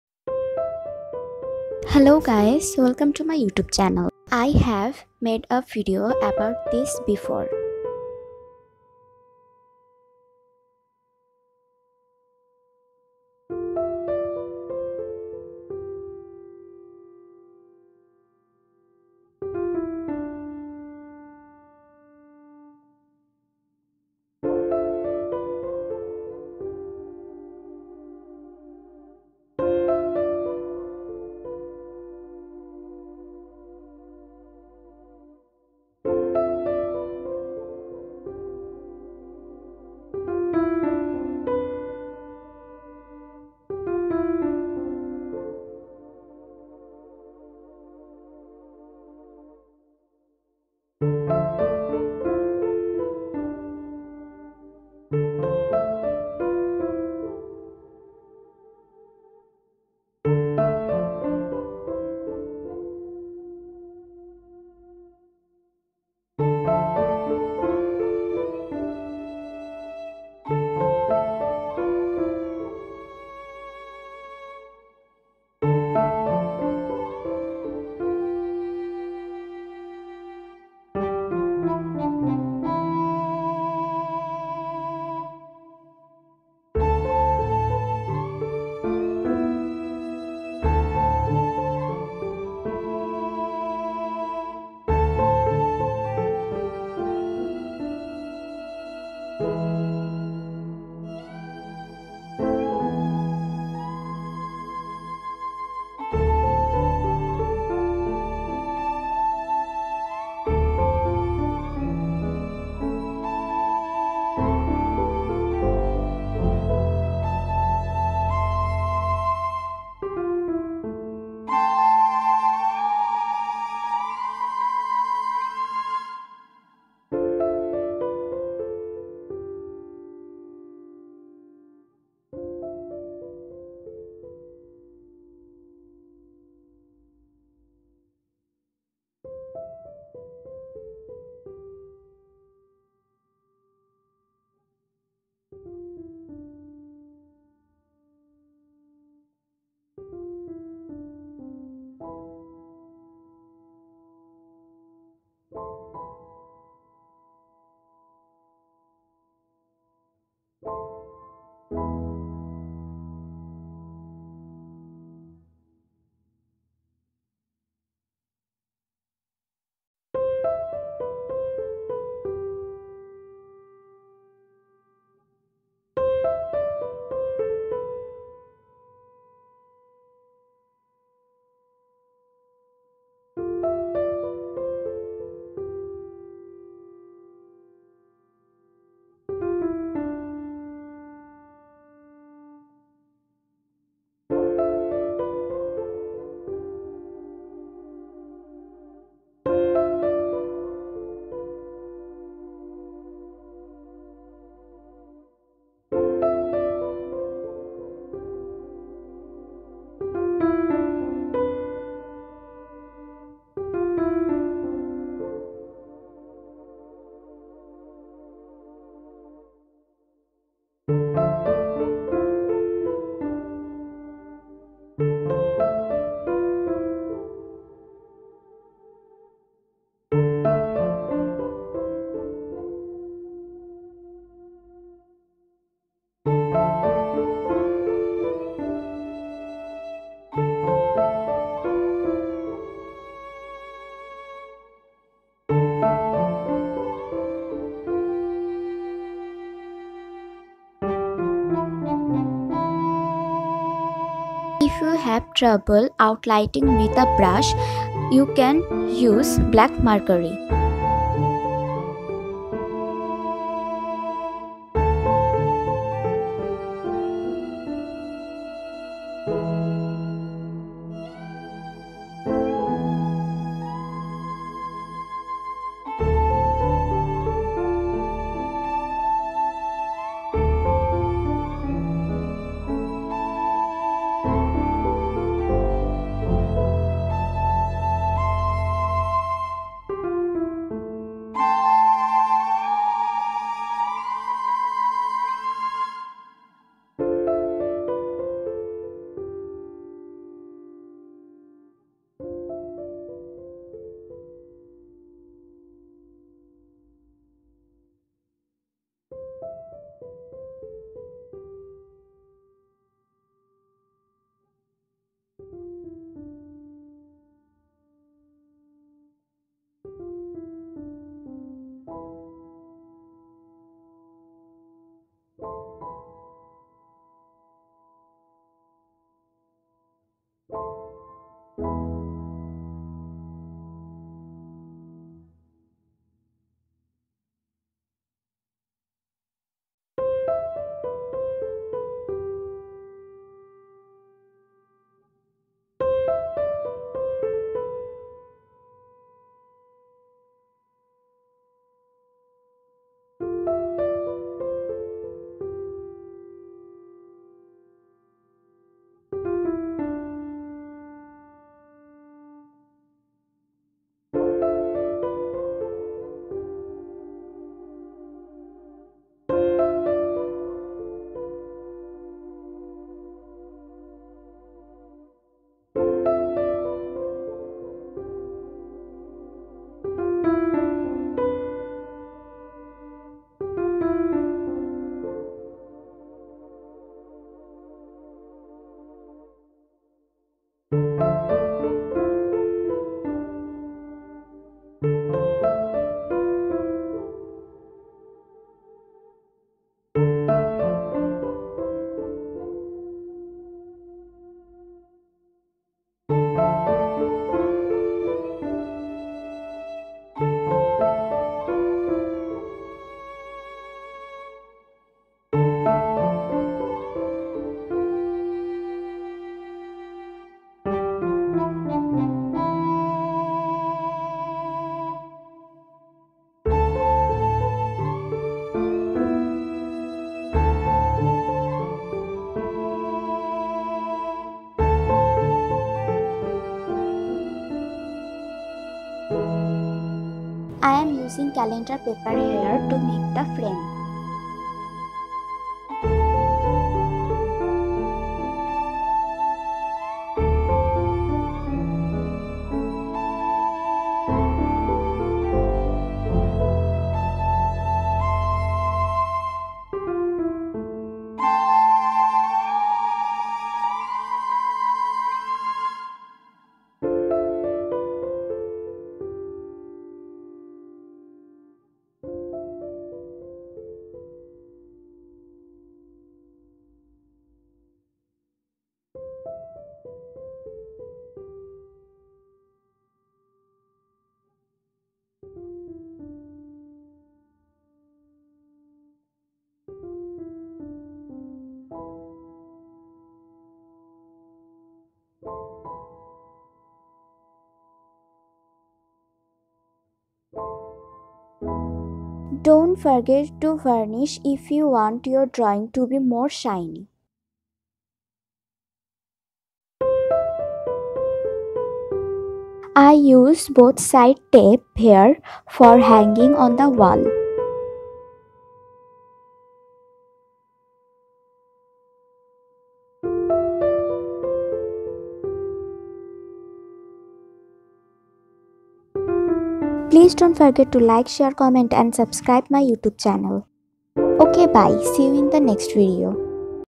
Paint A cardboard For Wall Hanging
In today's video I showed you how to paint a cardboard and fix it on the wall. I had made such a video before. You can see it if you want. Here is the link :

Using materials : 1. Cardboard
                         2. Acrylic colour
                         3. TiTi brush
                         4. Glue
                         5. Calendar paper
                         6. Both sided tape
                         7. Varnish
                         8. Anti cutter